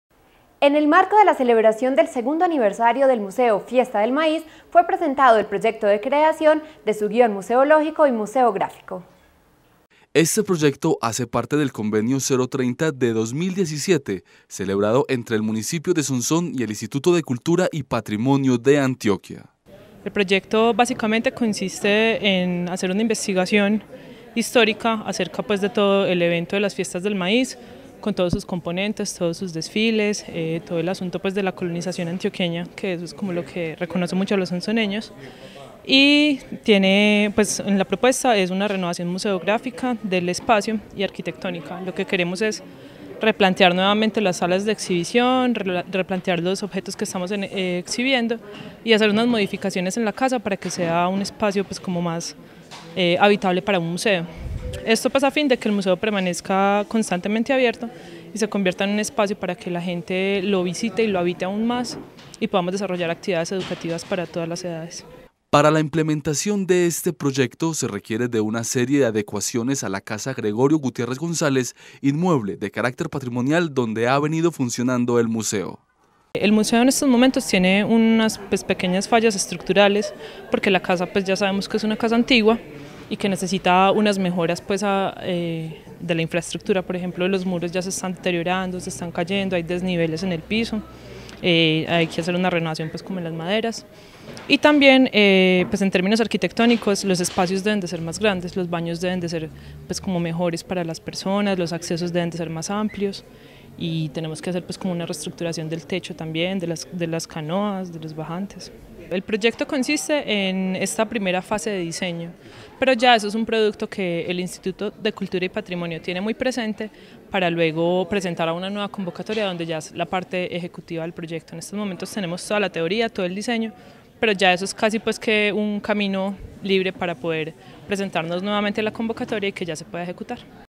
SEGUNDO ANIVERSARIO DEL MUSEO FIESTA DEL MAÍZ
En el marco de la celebración del segundo aniversario del museo Fiesta del Maíz fue presentado el proyecto de creación de su guión museológico y museográfico.